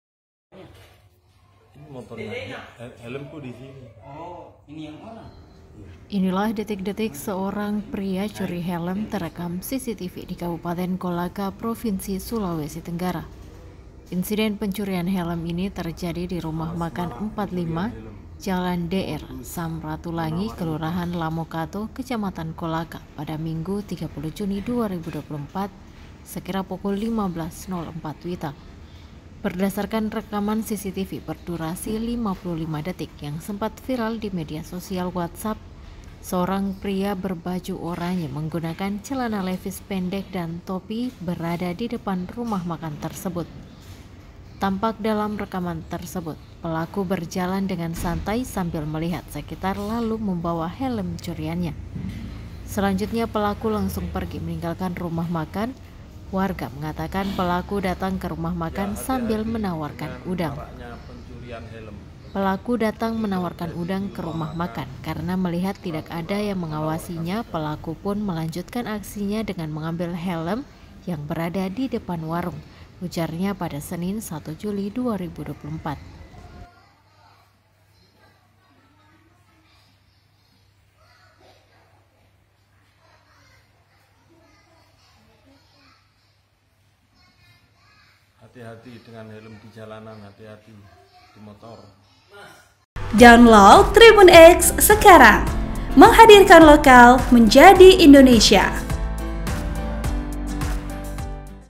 Detik-detik Pria Baju Orange Curi Helm Terekam CCTV di Kolaka Sultra, Pelaku Sempat Tawarkan Udang
Inilah detik-detik seorang pria curi helm terekam CCTV di Kabupaten Kolaka, Provinsi Sulawesi Tenggara (Sultra).

Insiden pencurian helm ini terjadi di Rumah Makan 45, Jalan DR, Sam Ratulangi, Kelurahan Lamokato, Kecamatan Kolaka, Minggu (30/6/2024), sekira pukul 15.04 WITA.

VP Wa Ode Ria Ika Hasana
Sumber Adrian